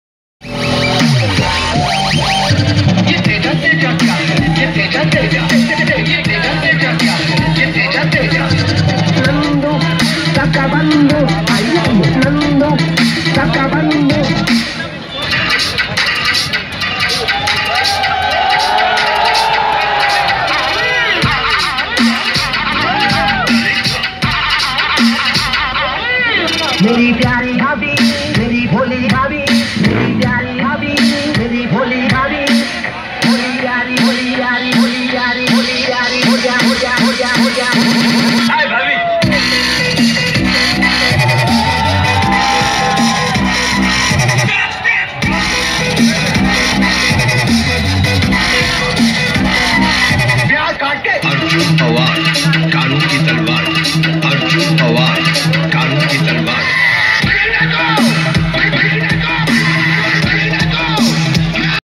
Faisalabadi boy performed well at Auto show Faisalabad | Traveling PK
Faisalabadi boy performed at Auto show Faisalabad. D Ground | Traveling PK